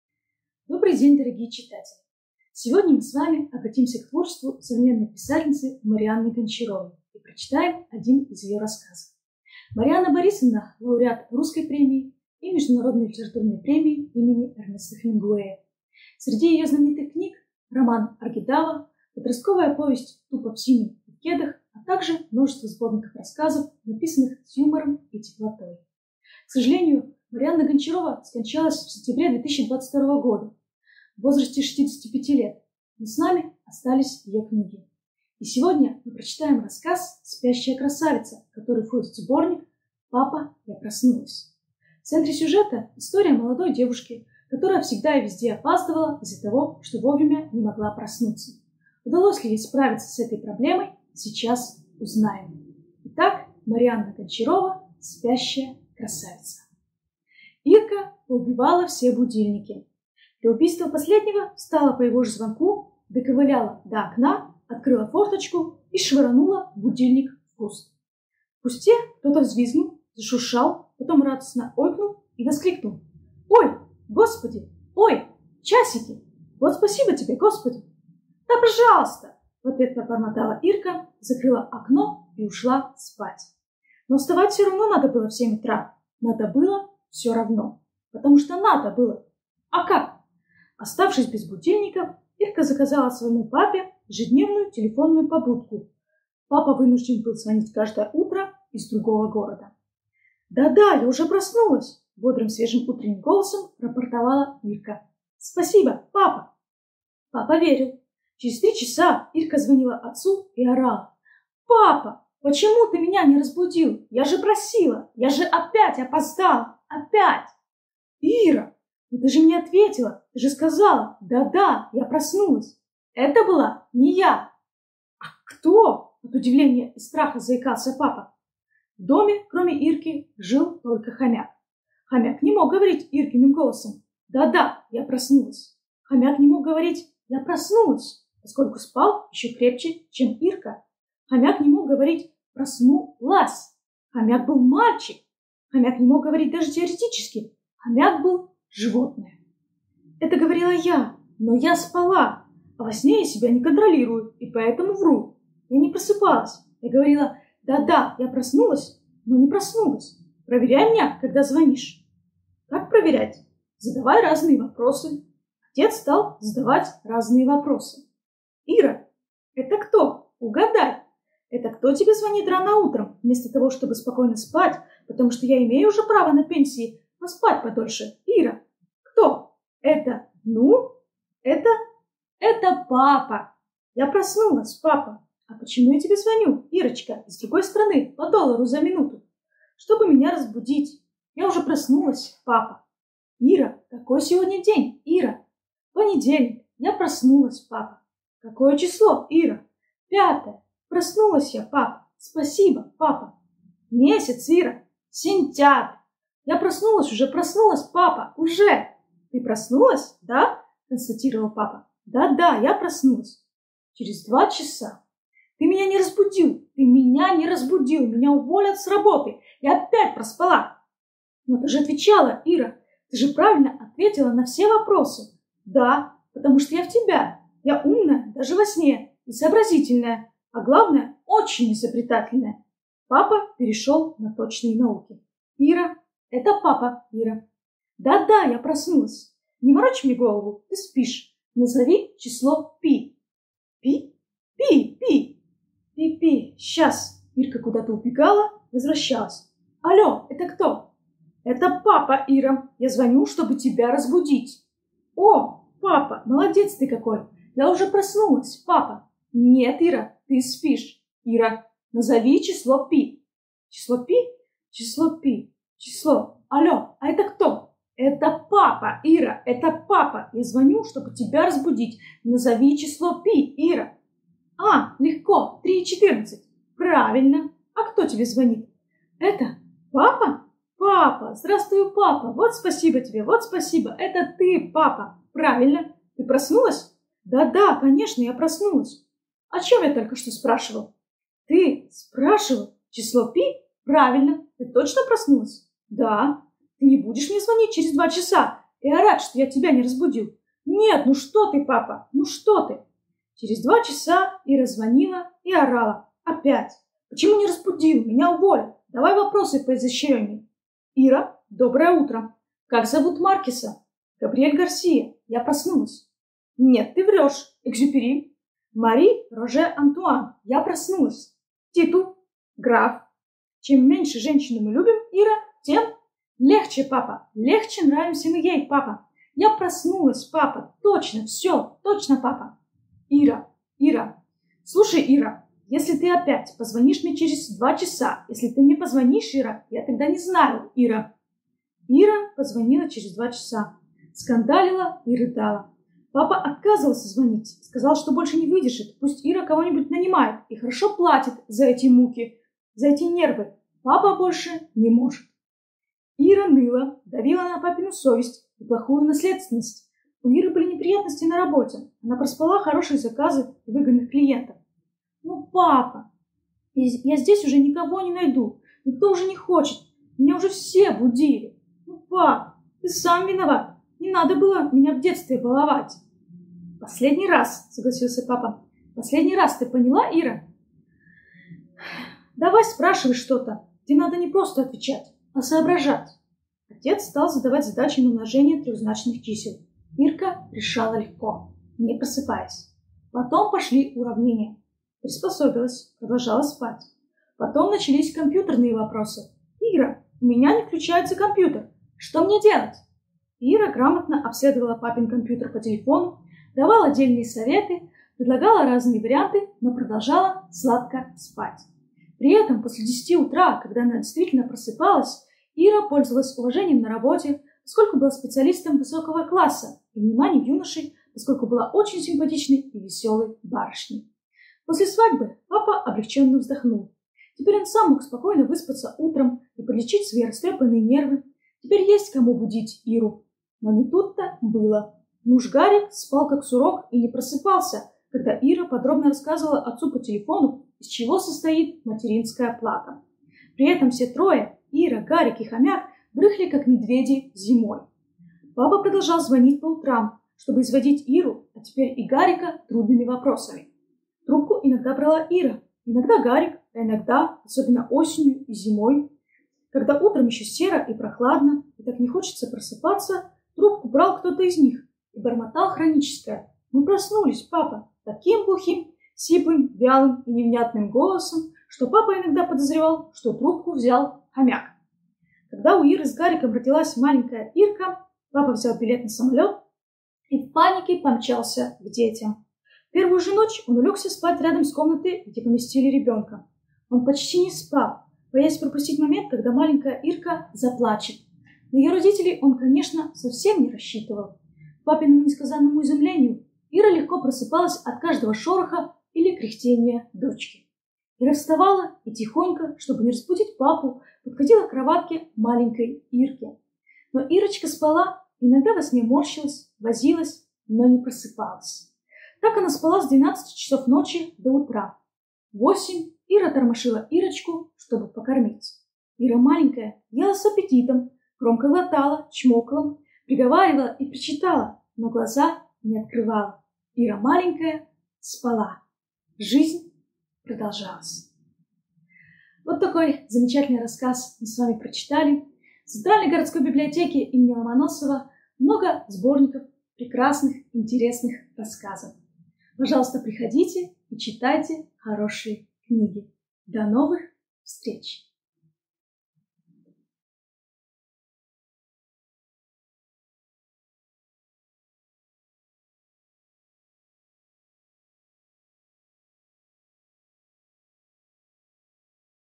Марианна Гончарова «Спящая красавица»
Центральная городская библиотека им. М. В. Ломоносова знакомит с  интересным рассказом современной писательницы Марианны Гончаровой. В центре сюжета — история молодой девушки Иры, которая всё время опаздывала на работу.